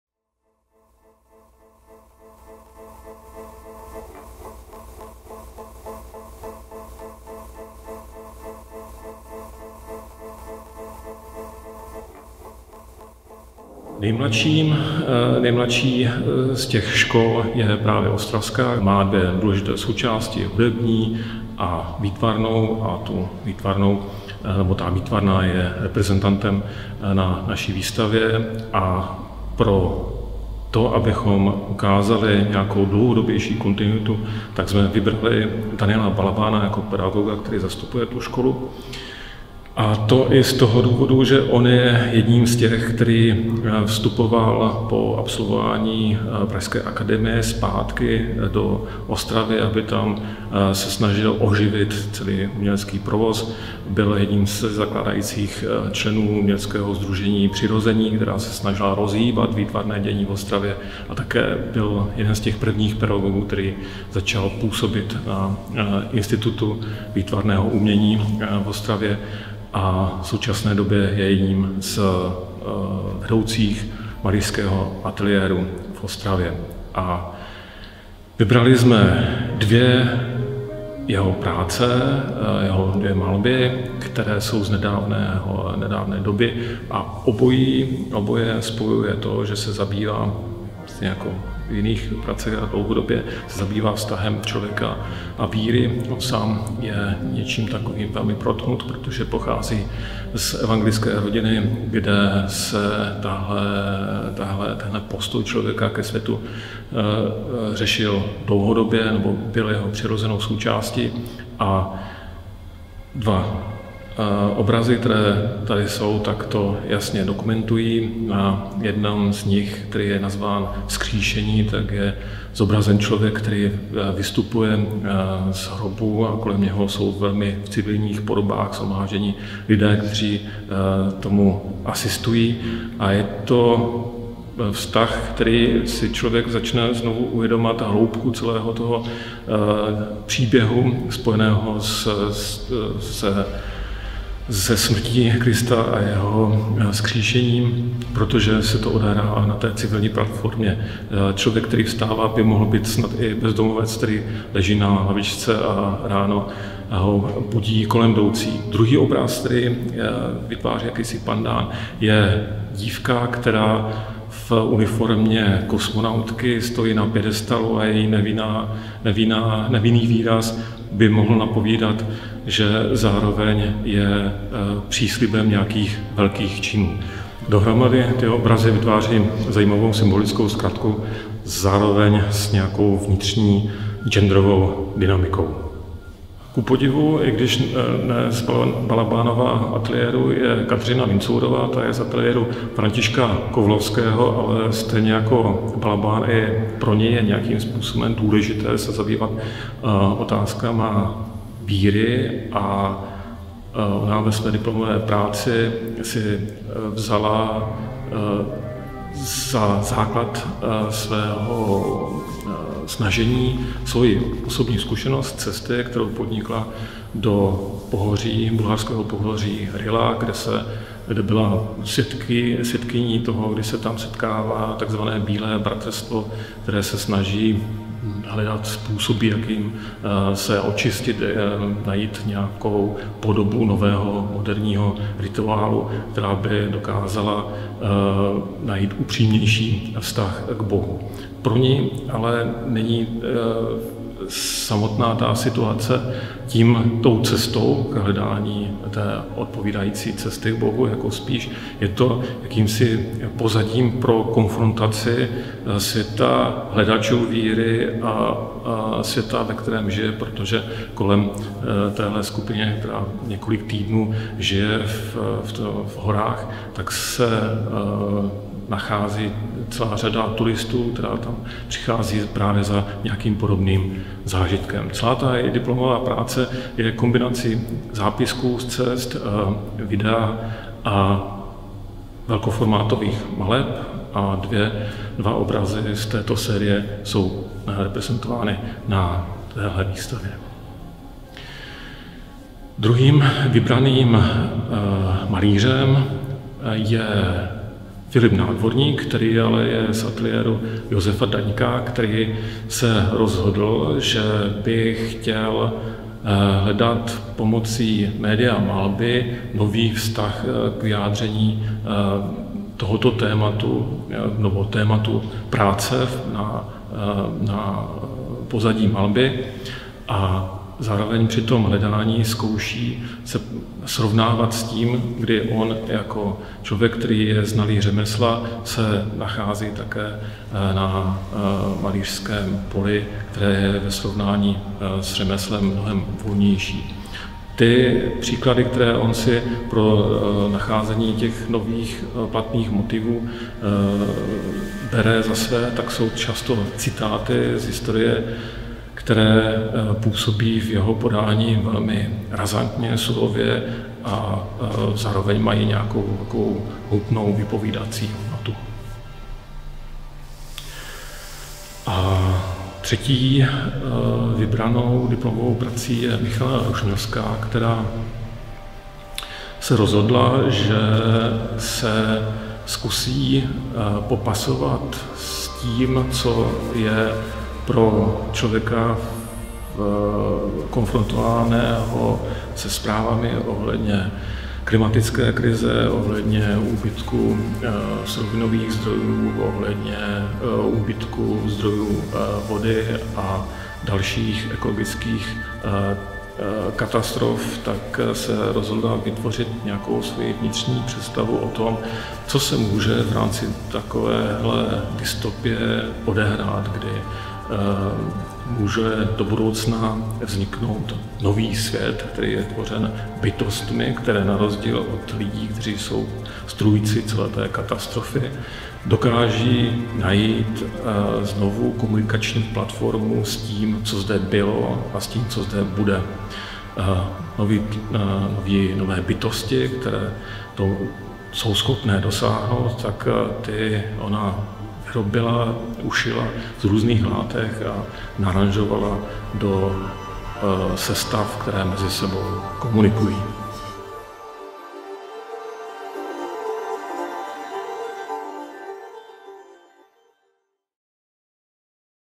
Umění volá! | Ostrava
Představujeme první díl ze sedmidílné série video-komentovaných prohlídek výstavy Umění volá!. První video je věnováno vystavujícím z Fakulta umění Ostravské univerzity. Celou výstavou Vás provede její kurátor Rostislav Koryčánek.

Výstava proběhla fyzicky v listopadu 2020 v Dům pánů z Kunštátu.

video: Martin Dominik Kratochvíl
grading: Ondřej Silný
titulky: Veronika Špundová